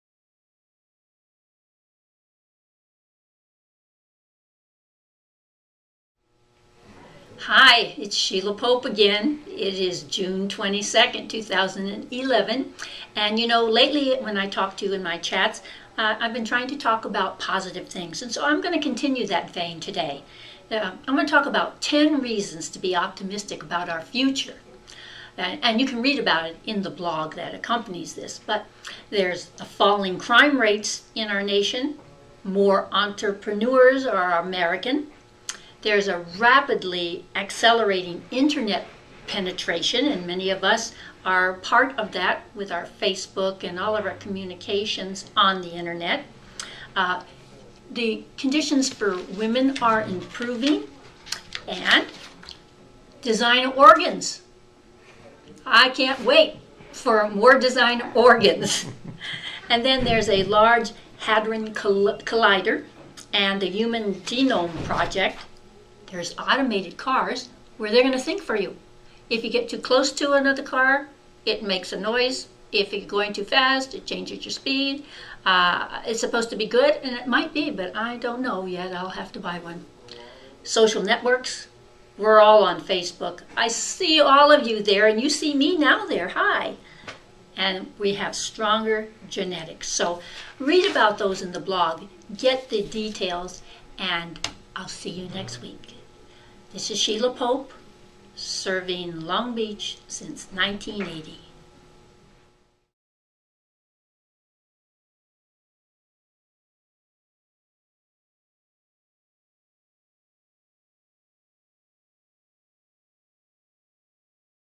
10 Reasons to be Optimistic About the Future
Recent polling data demonstrates that more and more Americans are uneasy about the future. There are plenty of problems on the horizon to keep us busy, but it's important to remember that the future also brings new innovations and opportunities. In no particular order, here are ten reasons you can be optimistic about the future.

• Falling Crime Rates
• More American Entrepreneurs
• Rapidly Accelerating Internet Penetration
• Improving Conditions for Women
• Designer Organs
• The Large Hadron Collider
• The Human Genome Project
• Automated Cars
• Stronger Genetics

for more details!

Sheila Pope
"Supporting equality in Long Beach"